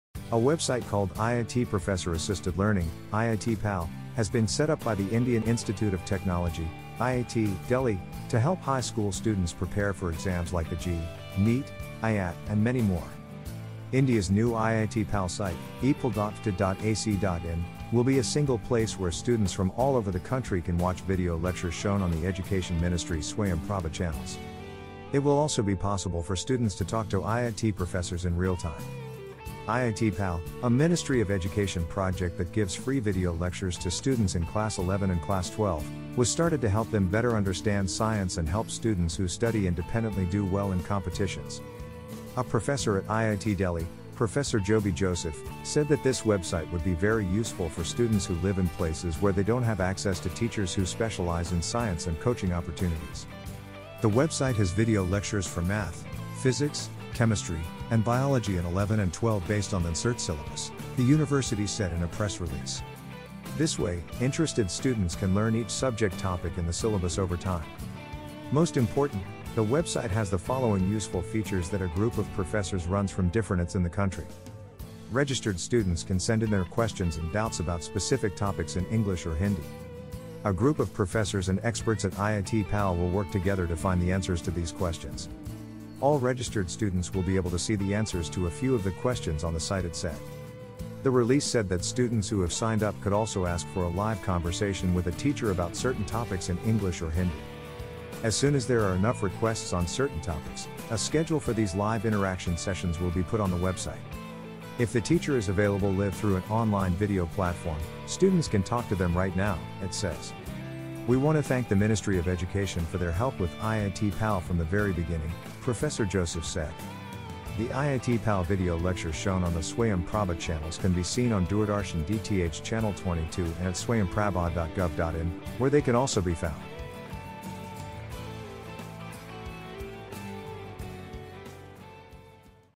Students can prepare for competitive exams with the help of IIT-PAL, by IIT Delhi
IIT-PAL is a project of the Ministry of Education that gives free video lectures to students in classes 11 and 12 for no charge.
The website has video lectures for math, physics, chemistry, and biology in classes 11 and 12 based on the NCERT syllabus.
This way, students can learn about each subject topic one by one as they watch the videos.
It will also be possible for students to talk to IIT professors in real-time.
IIT-PAL has video lectures for math, physics, chemistry, and biology in 11 and 12 based on the NCERT syllabus.
Registered students can send in their questions and doubts about specific topics in English or Hindi.
A group of professors and experts at IIT-PAL will work together to find the answers to these questions.
All registered students will be able to see the answers to a few of the questions.